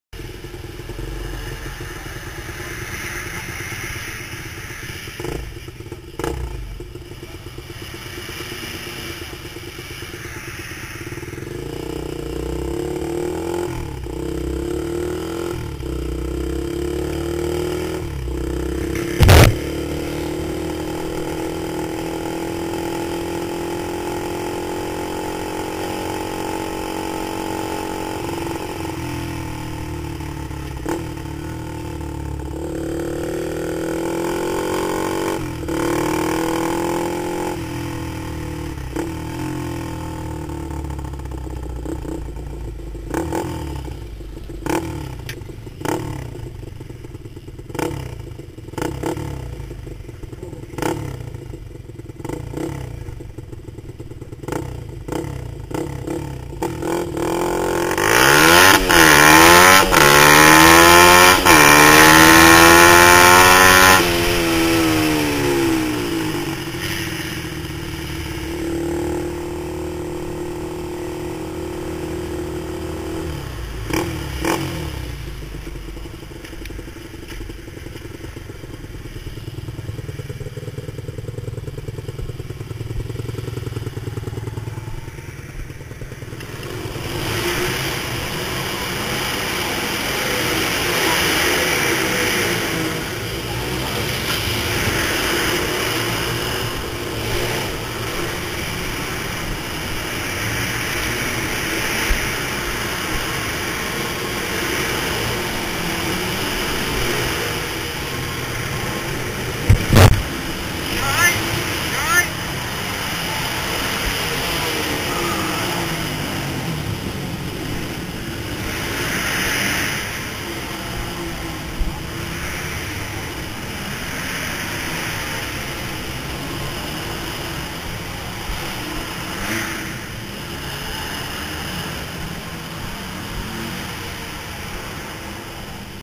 Allsports Grand Prix MiniMoto Crash 1/24/2010
This happened towards the end of B group as I was setting up my other camera for C group. Light rain was blown in by a breeze and made the track super slick causing this accident and two more before we figured out it was the rain not oil from a bike. Nobody was hurt though and we were all back riding very quickly.